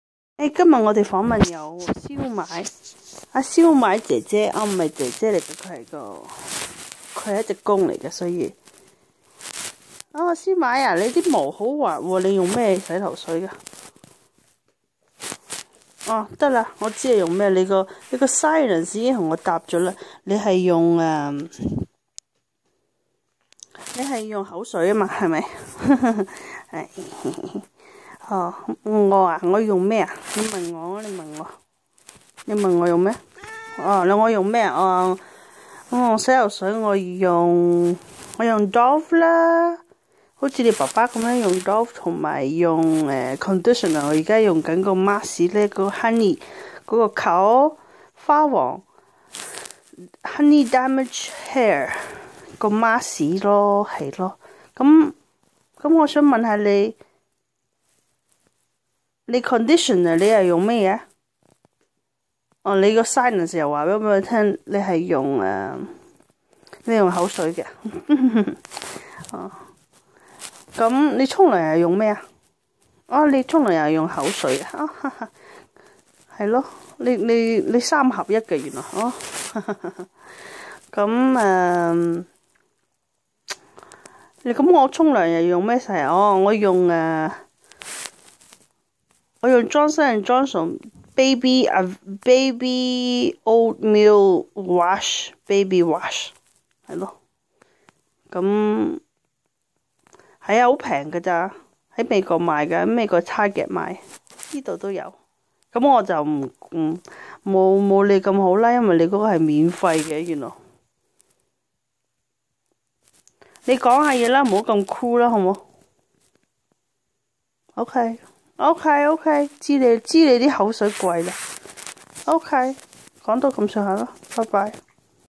Shampoo Interview w/ Siumai
Funny rant with our cat Siumai